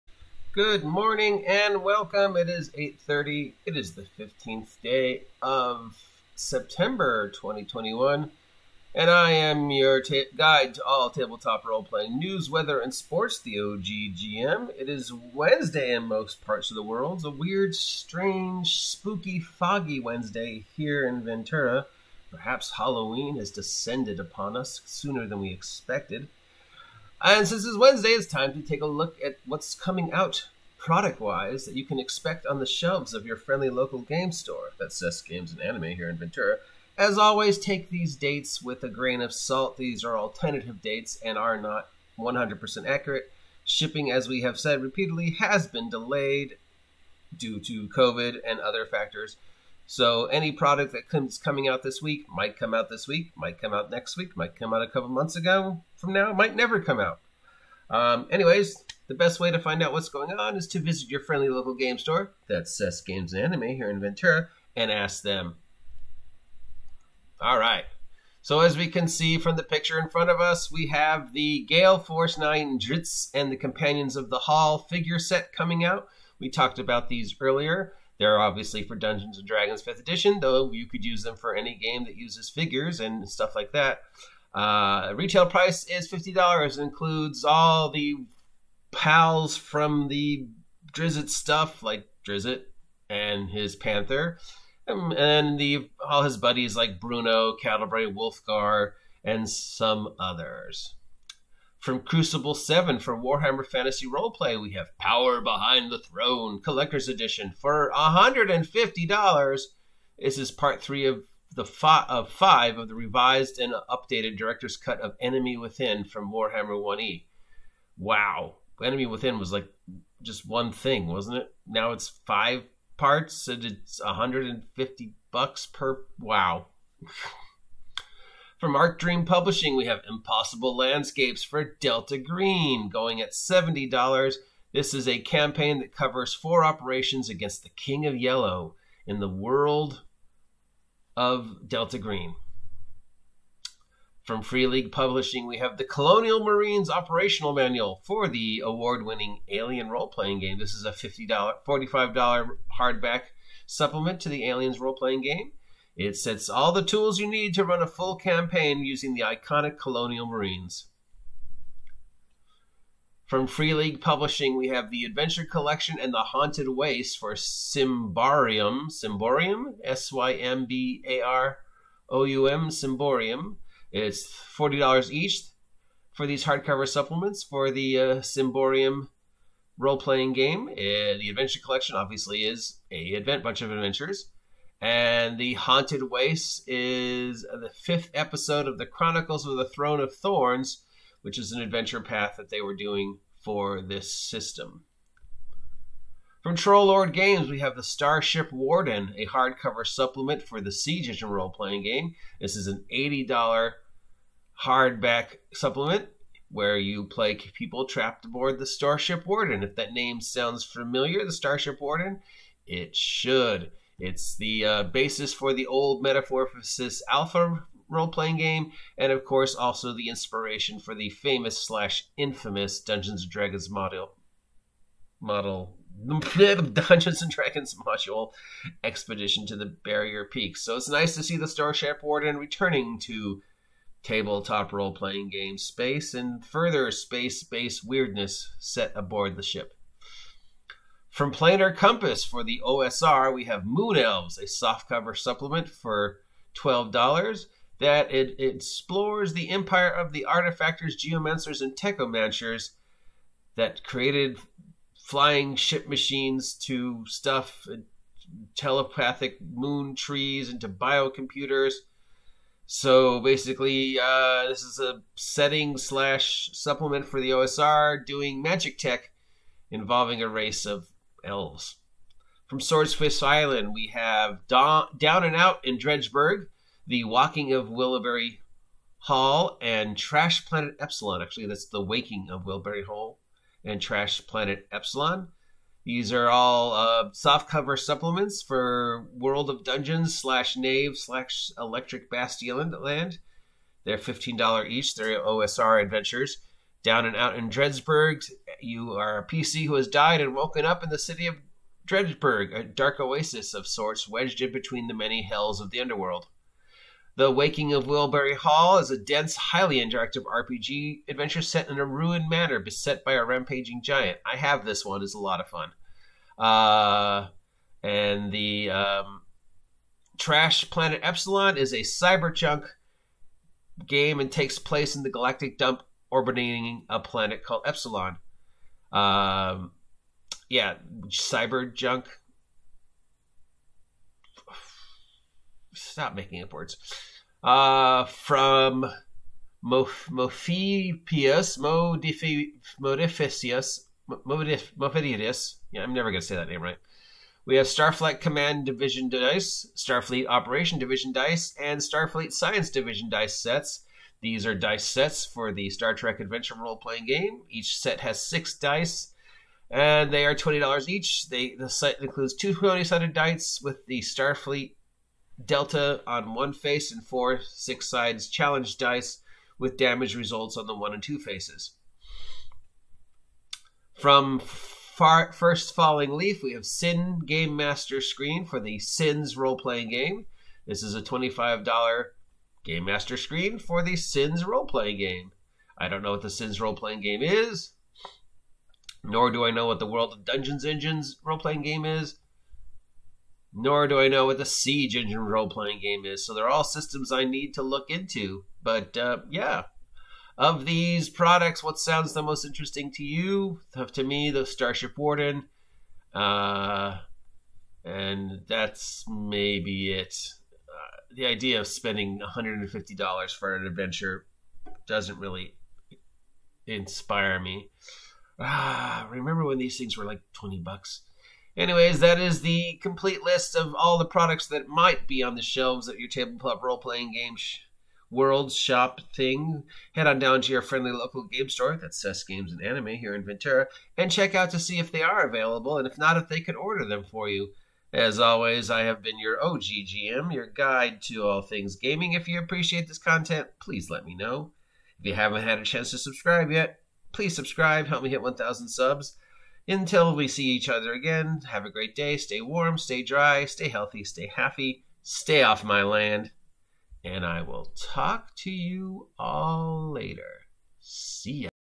Table Top Role Playing Print news of the week.
Here is this weeks selection of TTRPG games and supplements that will be on the shelves of your FLGS
"Friendly local game shop"